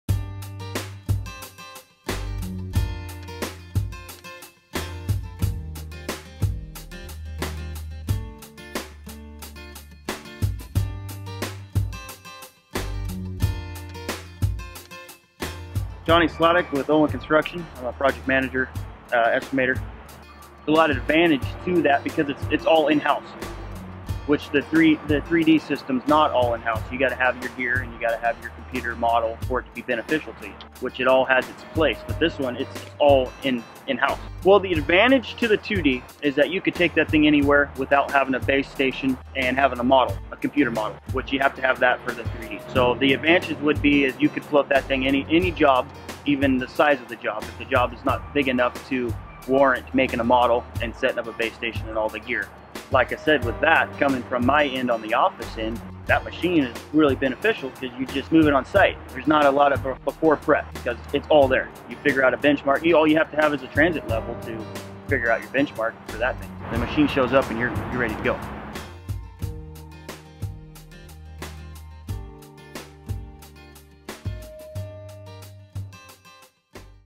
My Perspective on Cat® GRADE with Slope Assist™ | Johnny Sladick
Project Engineer Johnny Sladick shares his observations about the advantages of Cat® GRADE with Slope Assist™, which requires no external hardware, software or GPS signal.